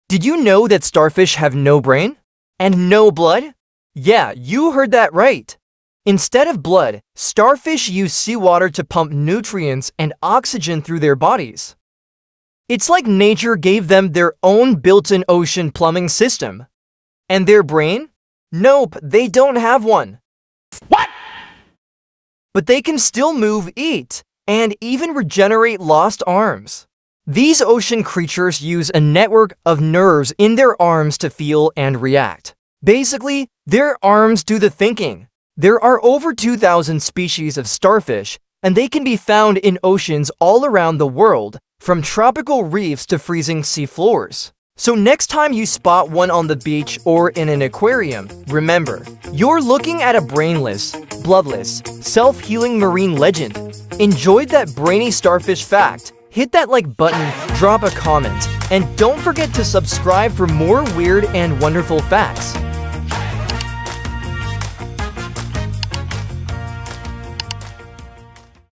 No Brain, No Blood—Still Thriving!🧠❌| Starfish Secrets Revealed!
Did you know starfish have no brain and no blood? 🤯 These incredible sea creatures pump seawater through their bodies to survive! Dive into this ocean wonder and discover why starfish are truly one of nature’s most bizarre creations. 🌊⭐